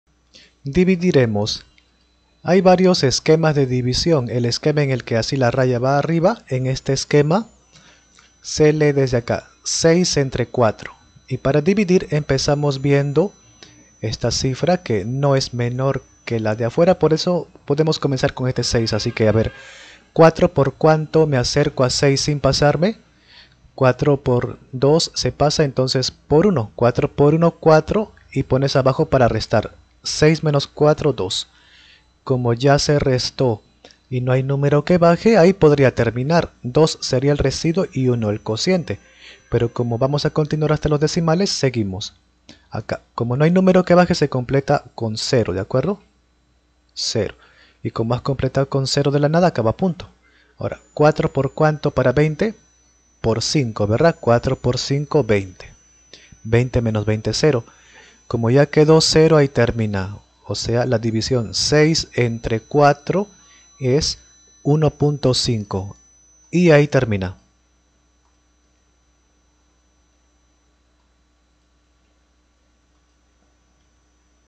Dividir 6 entre 4 division inexacta con resultado decimal de 2 numeros con procedimiento
division cociente residuo divisiones